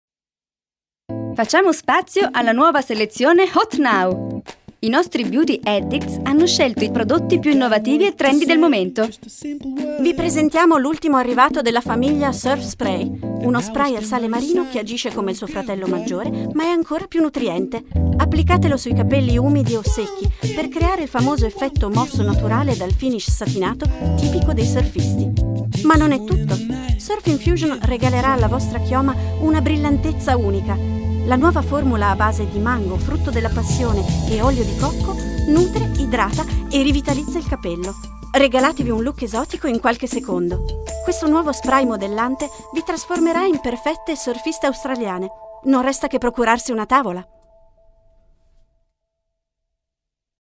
Sephora Hot Now - Bumble&bumble Surf Infusion - Quarta stagione 2015
Regalati una chioma brillante e mossa con lo spray Surf Infusion!
Scopri subito il suo formato mini, perfetto da mettere in valigia